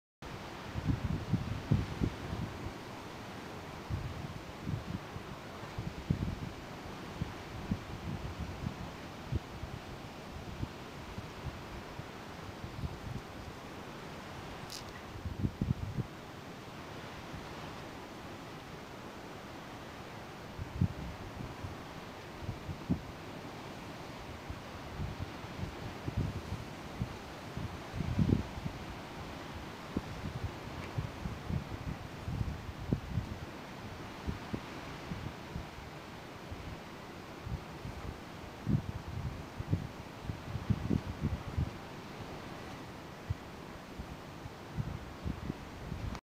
Bravo Hotel: Philosophiestunde
Sein ist wichtiger als Tun. "Die Qualität des Seins bestimmt die Qualität des Tuns. Das Tun muss auf dem Nichtstun basieren", liest man beim Zen-Meister Thich Nhat Hanh. -- Wie aus einem Lehrbuch für UN-Soldaten.
(UN-Stützpunkt 22, Golan, 28. März 2009)